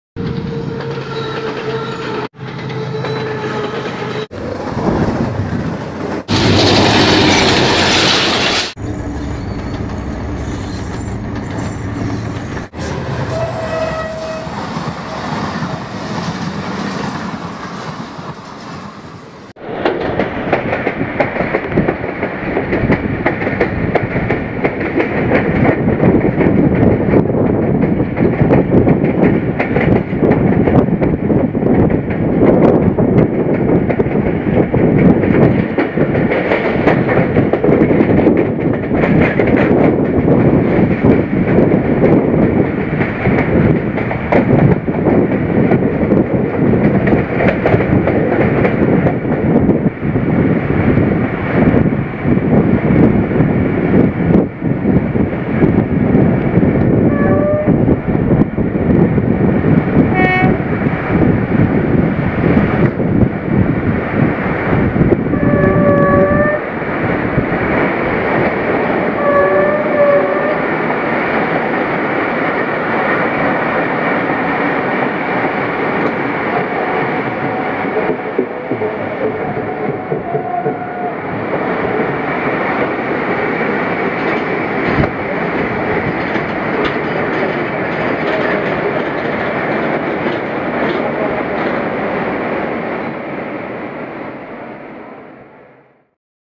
First Day|| 129 Up Thal Express meets 128 Dn Mehar Express at Muzaffar Garh Railway Station|| 07:35
Date: 13-02-19
Locomotive: PHA-20 8318